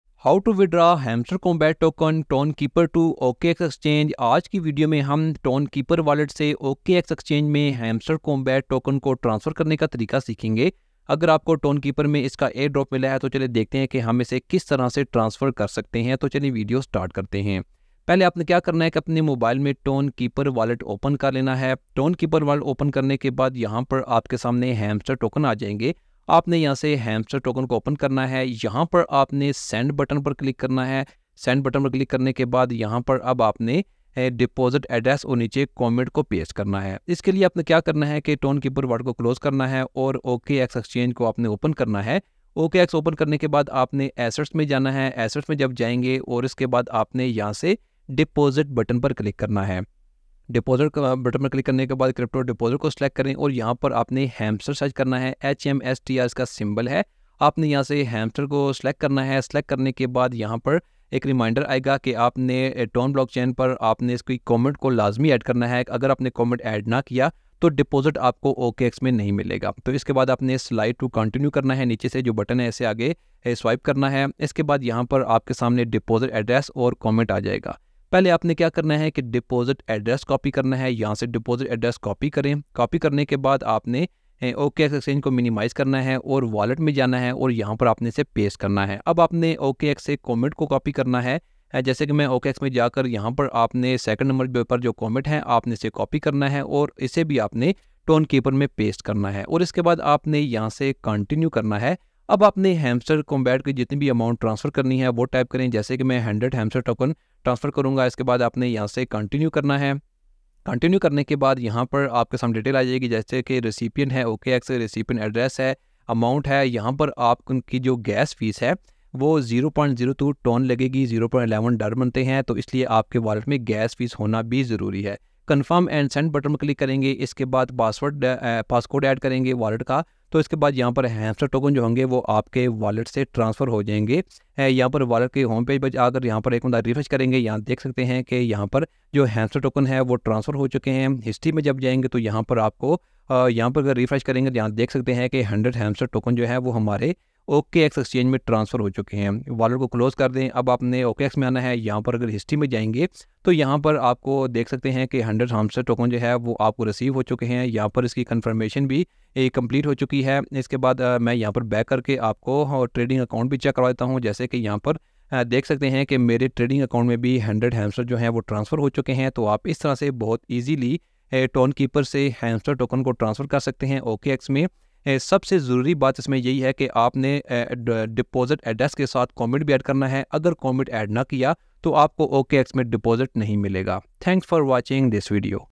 Hamster Withdraw Tonkeeper Wallet to OKX | How to Send Hamster Kombat from Tonkeeper Wallet to OKX
In today's video, we have learned how to transfer Hamster Kombat tokens from Tonkeeper wallet to OKX exchange. If you have received Hamster Kombat airdrop in Tonkeeper wallet and you want to transfer it to OKX exchange and cash it in your bank from there then in this video we have learned how to transfer Hamster Coin. Can in OKX exchange...

Cover Topic:
how to send hamster coin tonkeeper to okx
how to transfer hamster kombat tonkeeper to okx